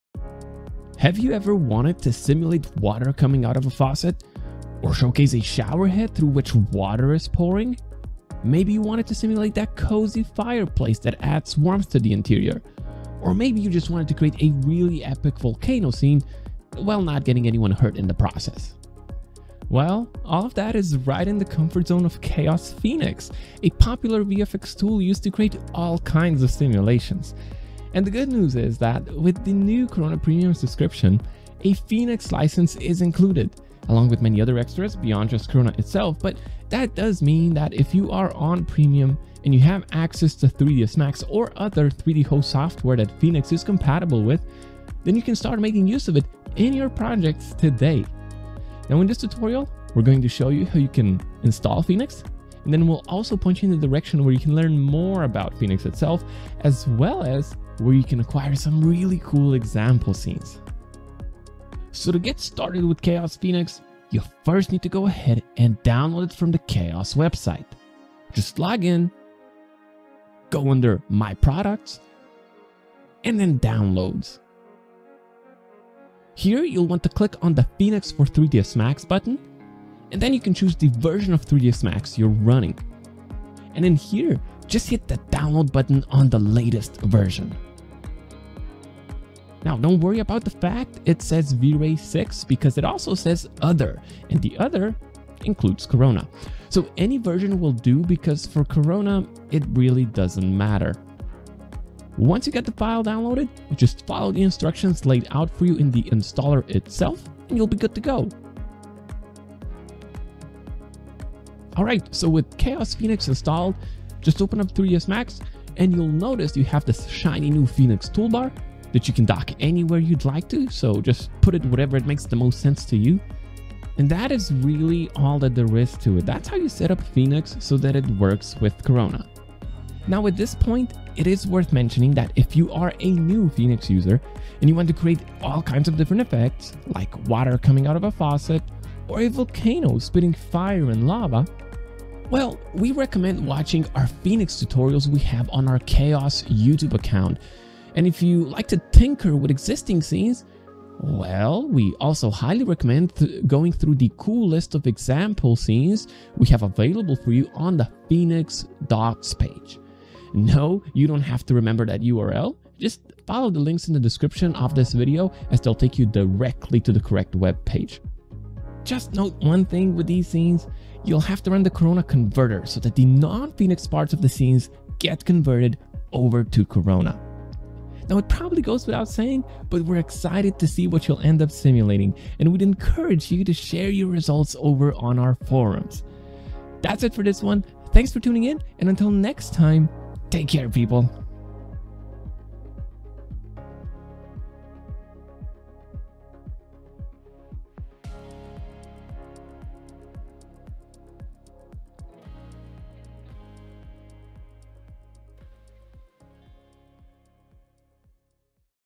Installing Chaos Phoenix for Corona in 3ds Max
Chaos Corona (Premium subscription, for 3ds Max) now ships with Chaos Phoenix. Phoenix is capable of a wide range of effects including fire, smoke, liquids, flames, explosions, rigid body simulations, ocean waves, mist, and splashes to list just a few. Quick presets and fast setup make it easy to get started so that you can add pools, showers, faucets, fireplaces, and more to your stills and animations. Its powerful simulation engine also offers complete control over more complex effects too.

In this How To tutorial we'll be taking a look at how to get Chaos Phoenix installed on your system as well as where you can find additional resources on how to use it.